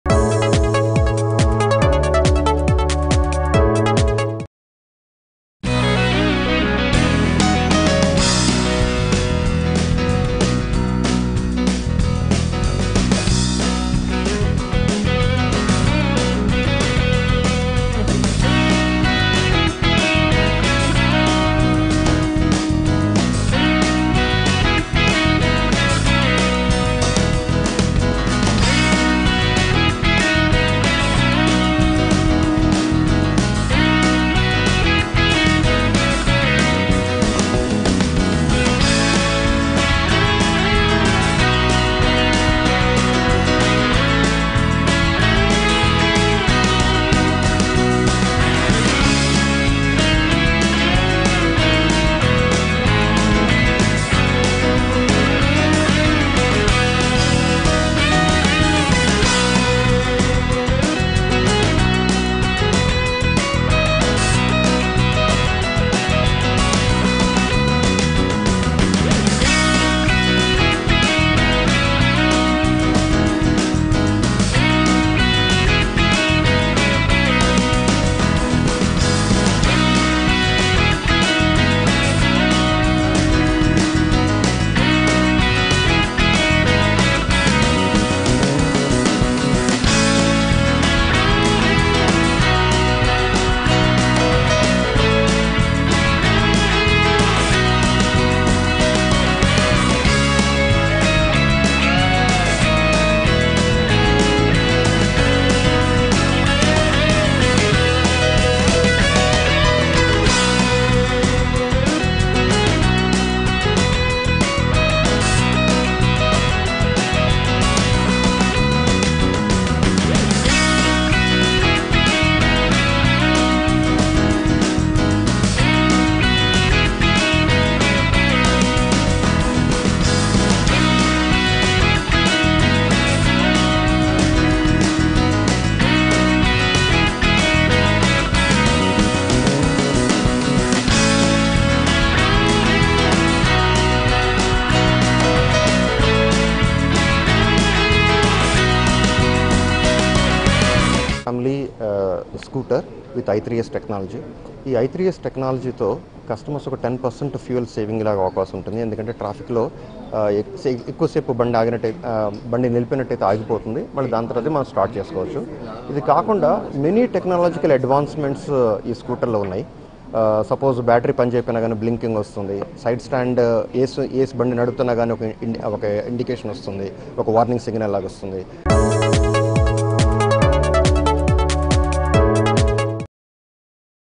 Hero Destini Bike Launch by Pullela Gopichand at Hyderabad | ABN Entertainment
Hero Destini Bike Launch by Pullela Gopichand at Hyderabad. Hero Destini Bike News and Updates Can Be Known Here.